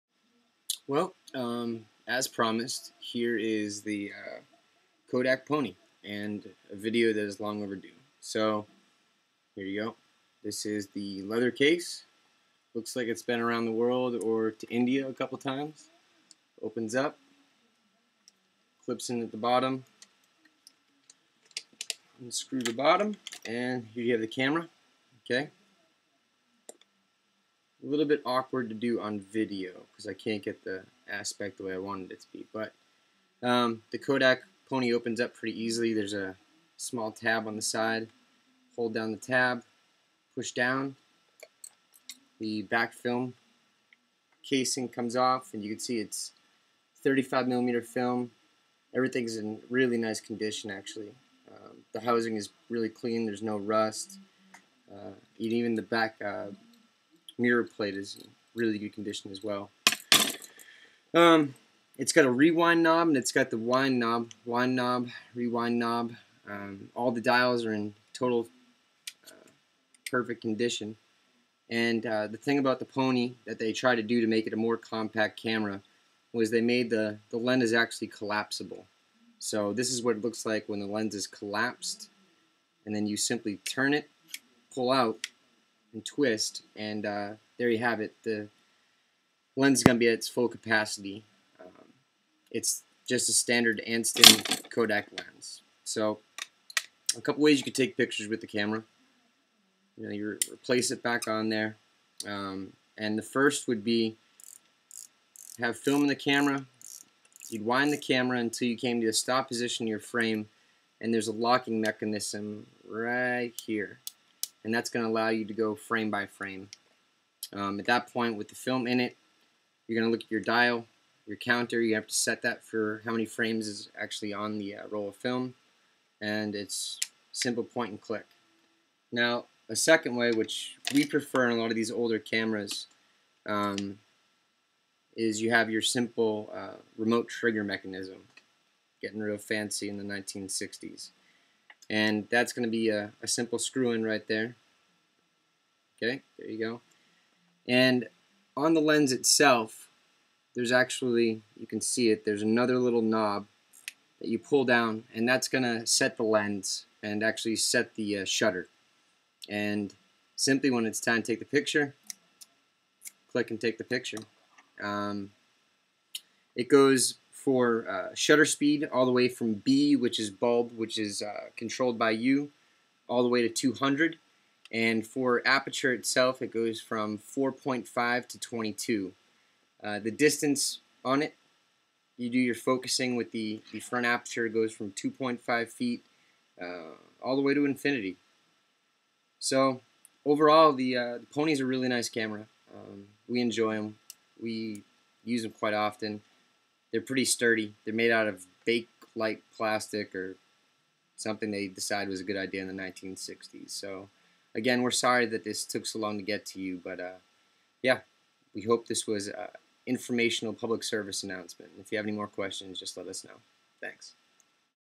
kodak.mov
Video explaining the workings of a Kodak Pony 135mm camera for a customer